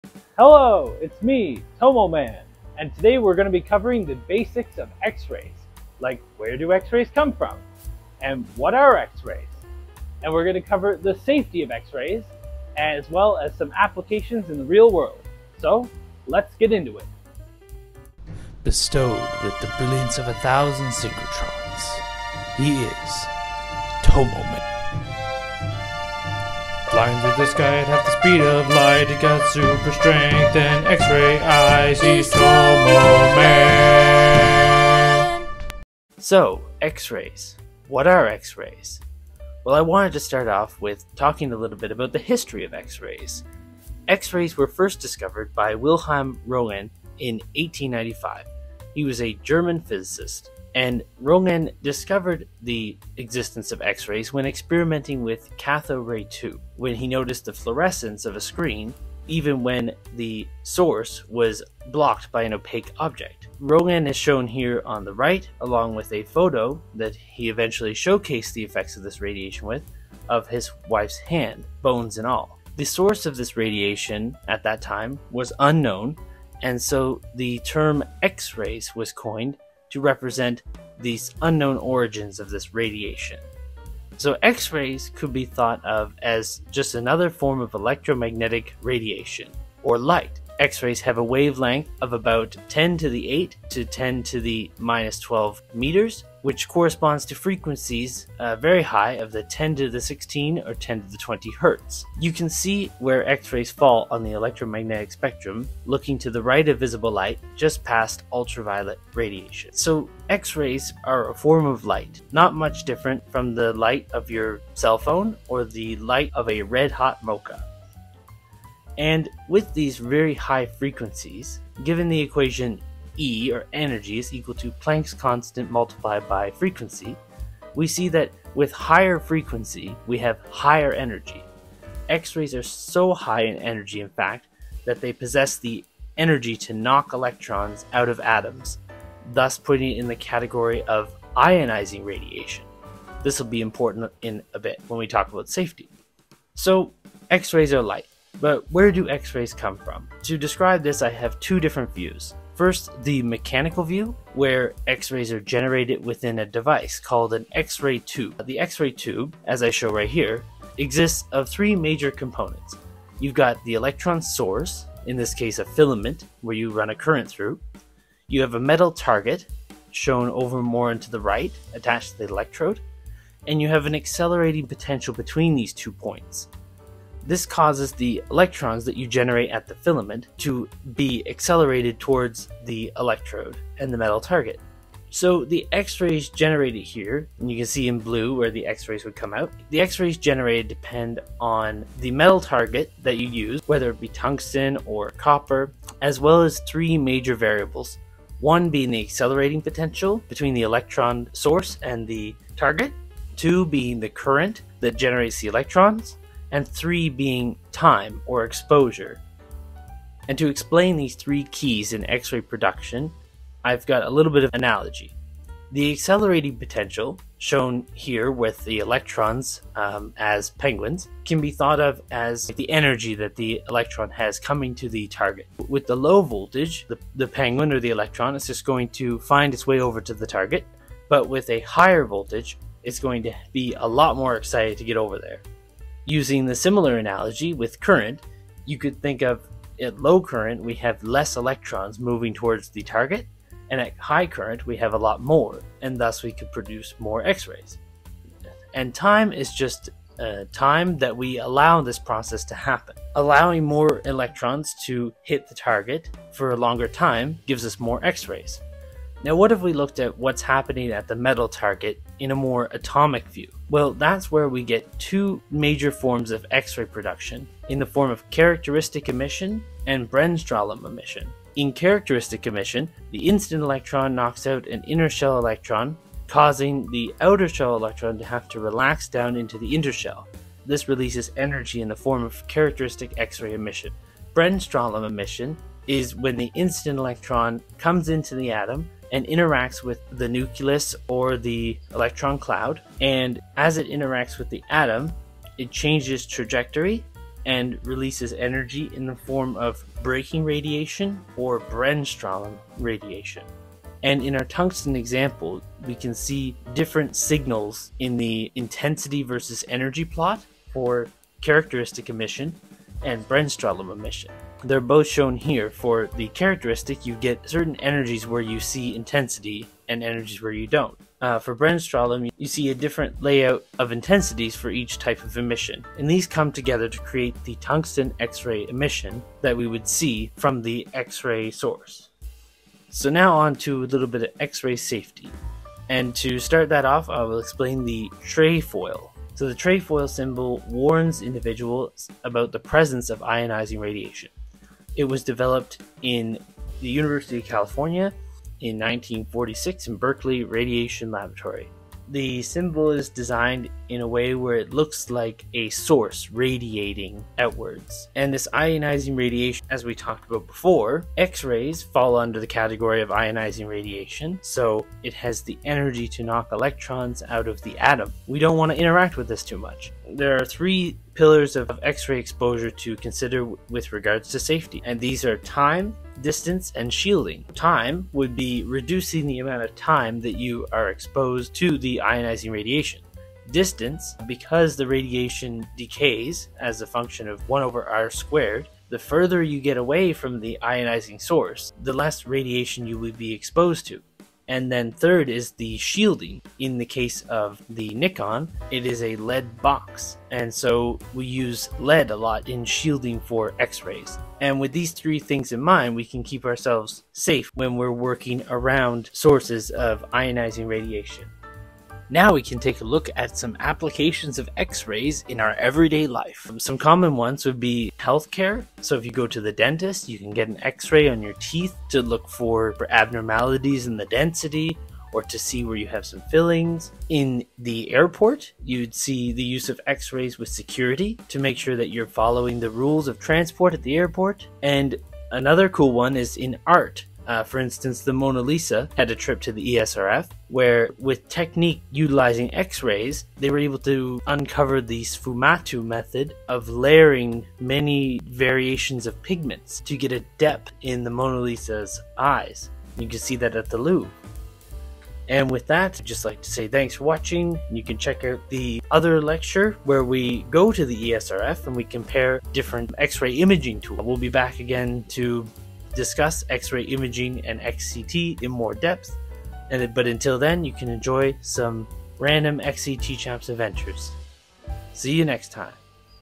The BASICS of X-RAYS! - Tomoman Lecture 2 - A brilliant EXPOSURE to X-rays
Special thank you to Isabel Antony for helping gather the information needed for this video, and apologies for every German word I attempted.

Dive into the captivating world of X-rays with Tomoman's latest lecture!
In this enlightening video, we take an exploration into the origins of X-rays, unravelling their mysterious journey from discovery to application.

Discover the fascinating science behind these penetrating rays and their remarkable ability to unveil hidden worlds. Tomoman sheds light on the crucial role of X-rays in society.

From safety insights to astonishing applications, this lecture promises to expand your knowledge and ignite your curiosity about the incredible world of X-rays!